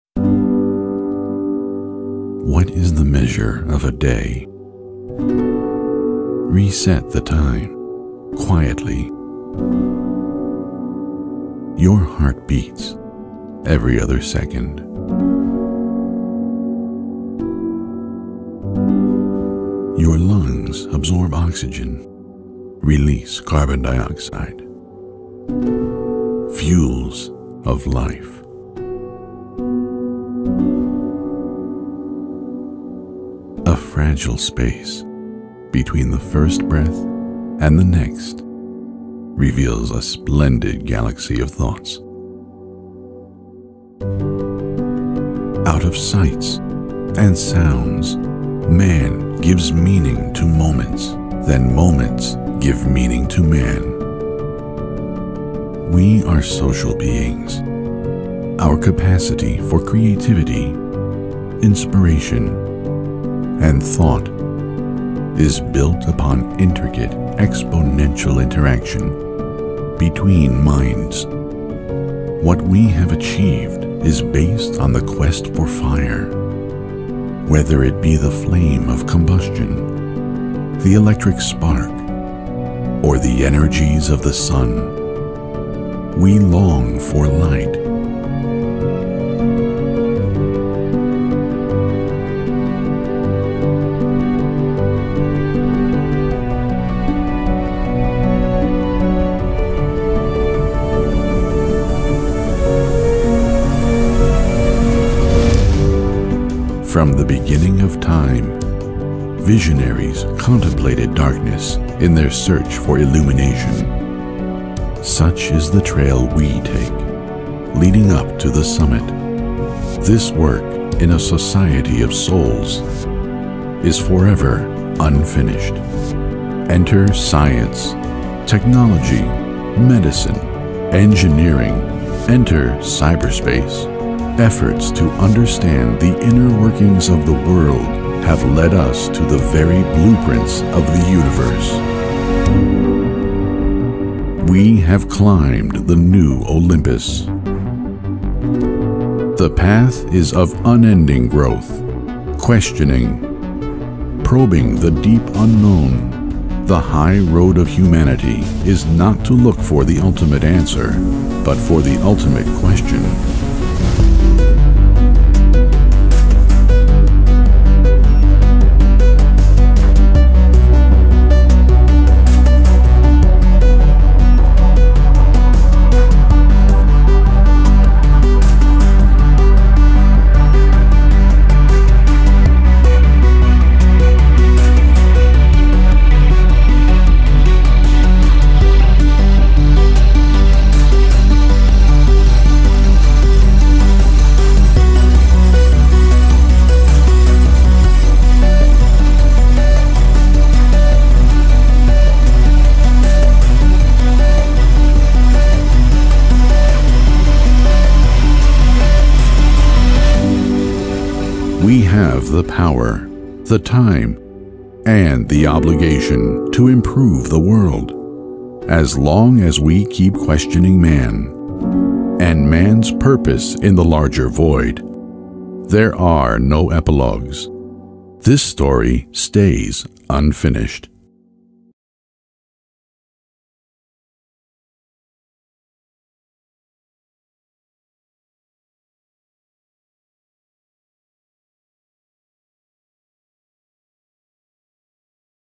UNFINISHED 2018 AFTER MOVIE
A look back at some of the stories started at UNFINISHED18 in Bucharest last September. Like our community, this short movie is ignited from serendipitous dreams and beautiful connections and demands a little bit of the thing we value the most: TIME.

Air Date: July 27, 2019
Cinematography by: Marius Panduru
Edit by: Marin Dinescu
Directed by: Cristian Movila
Voiceover:  Ken H.
Text by: Gabriel D.
Special thanks to:  Viorel Chesaru, John Florescu - ChainsawEurope Studio